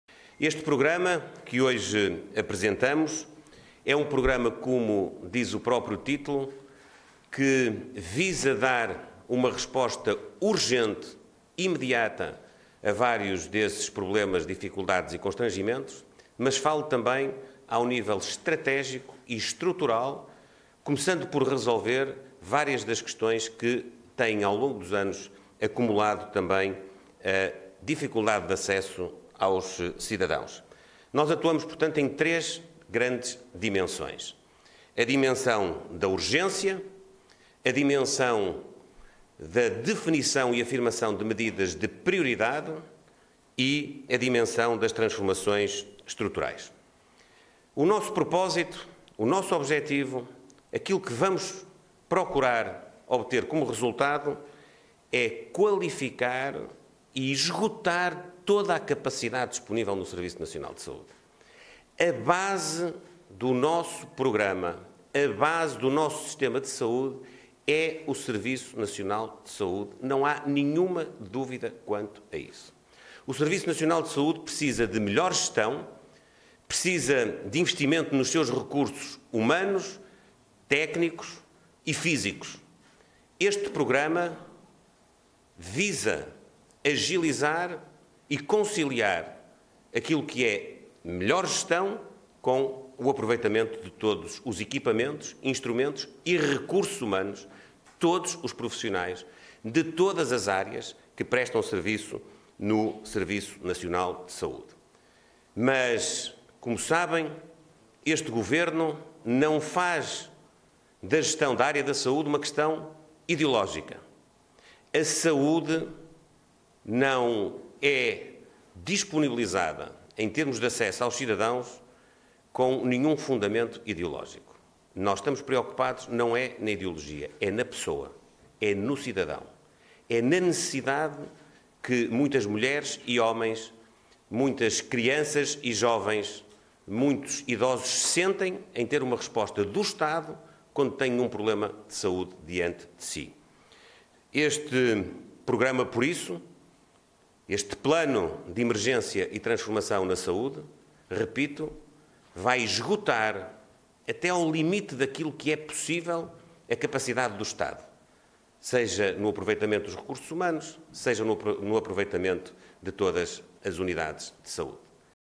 Plano de emergência para a saúde prevê esgotar a capacidade do SNS até ao limite
O primeiro-ministro anunciou que o programa de emergência para a saúde pretende esgotar os recursos do Serviço Nacional de Saúde (SNS) até ao limite, mas conta também com os setores social e privado de forma complementar.

Edição de Ana Folhas